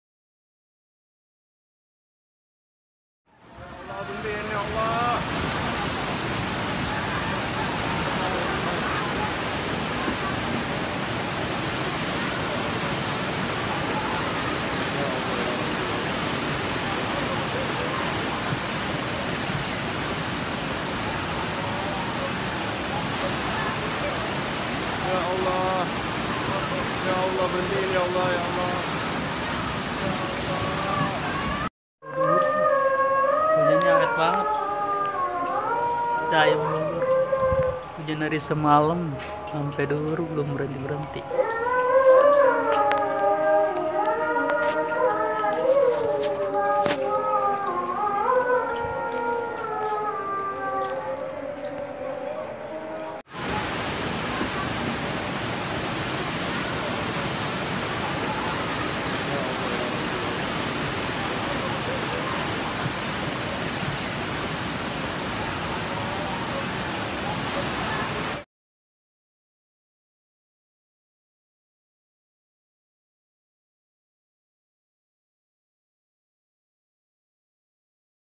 Banjir Bandang Tanggul Kali Jebol Bencana Banjir Awal Tahun Baru 01-01-2020 Bojong Gede (Bogor)
Semoga Dari video ini Ada pihak pihak yg perduli Menangani Dan memberi solusi perbaikan perbaikan Agar kejadian tidak terulang.